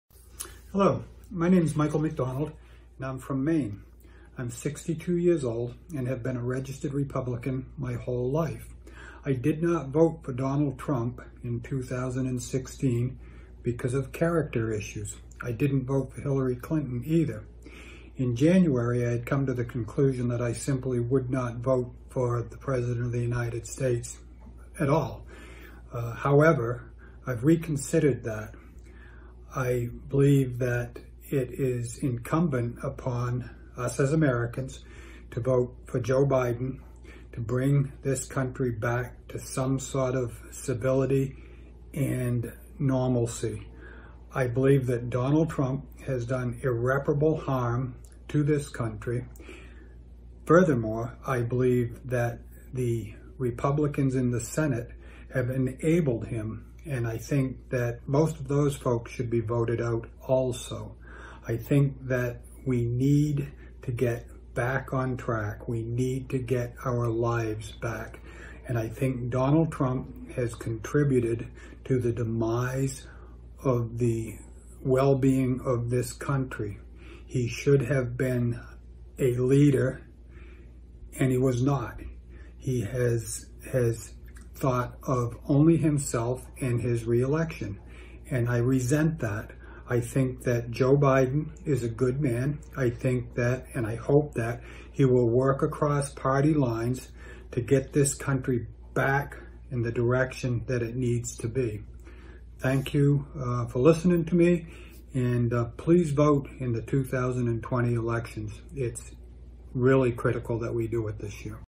Don't just sit on the sidelines in 2020, says this Republican voter
"I believe that it is incumbent upon us as Americans to vote for Joe Biden to bring this country back to some sort of civility and normalcy."